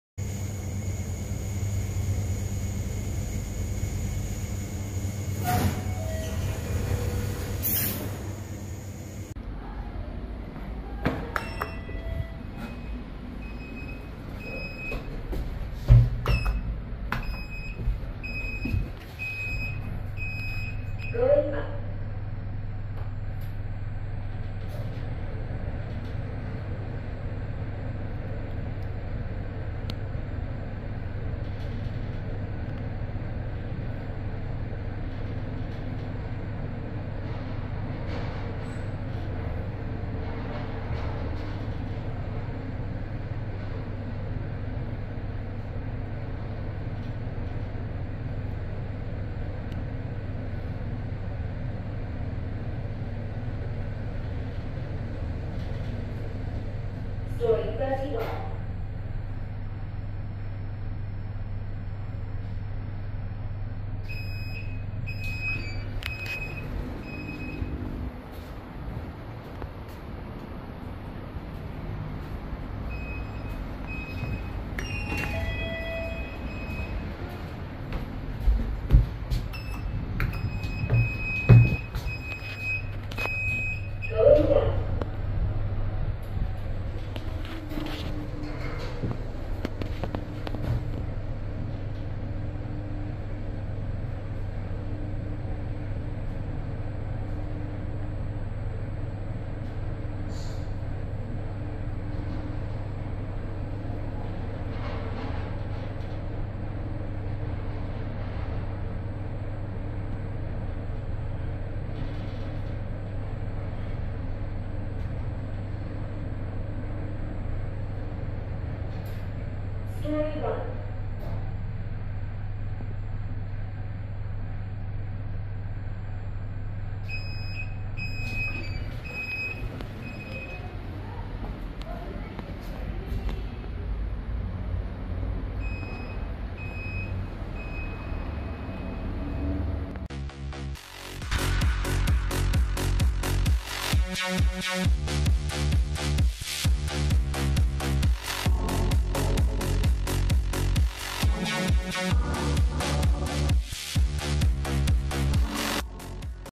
EM lifts at Blk 633B Senja Road
Amazingly high-speed EM lifts! Although it mocked GEC Express lifts in a way where the floor indicator shows accelerated instead of actual position. Original thin arrows on this indicator would have been better in my opinion.

Manufacturer: EM Services/Shenyang Brilliant (BLT)
Floors: 1-31
Capacity: 13 persons 885kg
Year installed: 2011
Rating: 45/50
History: Original

Camera: Samsung Galaxy S23 Ultra
Date filmed: 27 May 2023